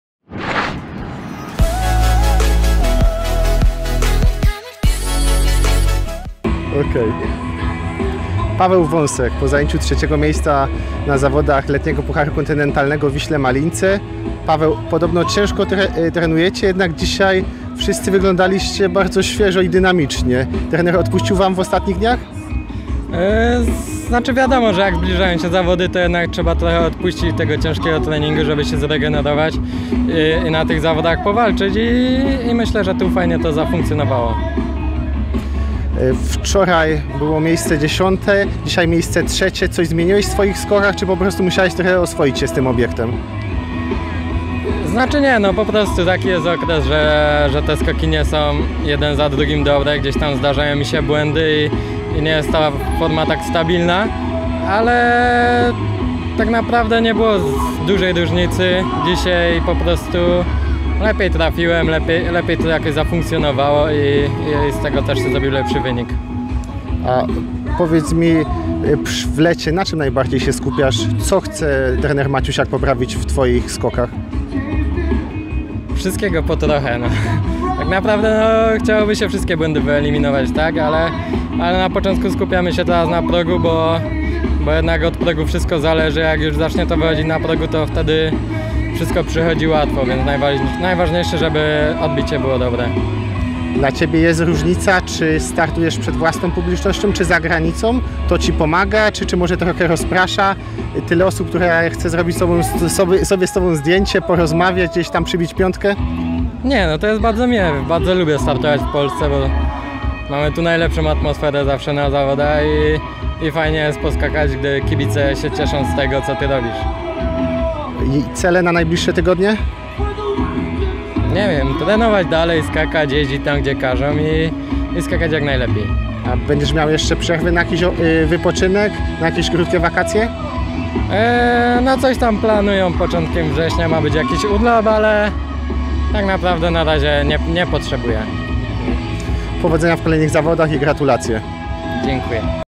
Paweł Wąsek na podium: Przerwa od skoków? Nie potrzębuję urlopu
Paweł Wąsek po zajęciu trzeciego miejsca w Pucharze Kontynentalnym w Wiśle  (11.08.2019 r.)